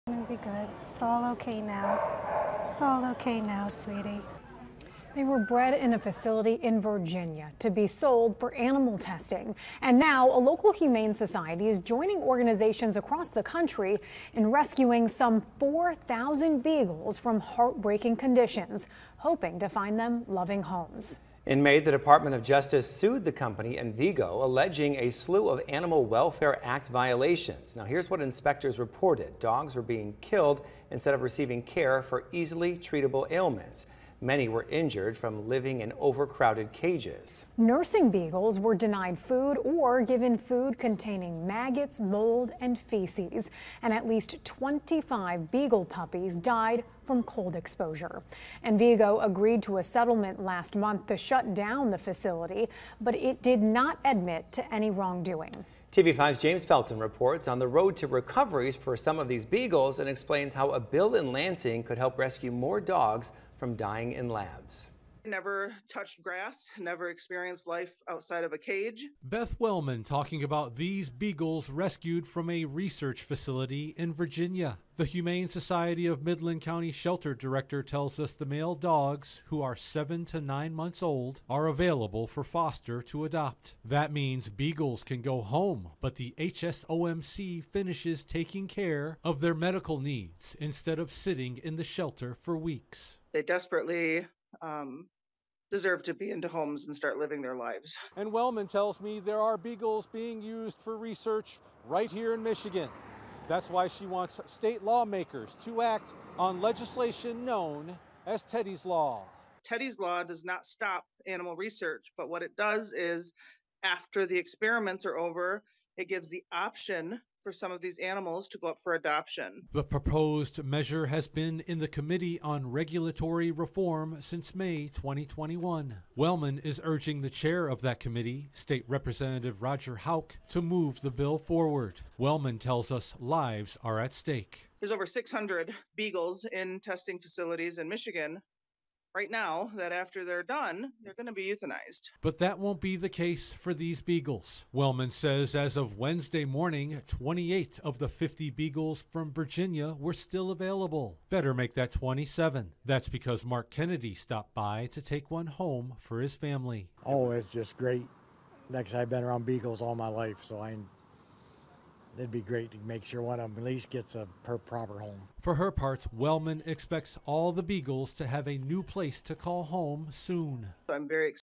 Humane society looking for homes for beagles rescued from animal testing lab
A local humane society is joining other organizations from around the country to find a forever home for some of the 4,000 beagles rescued from the awful conditions.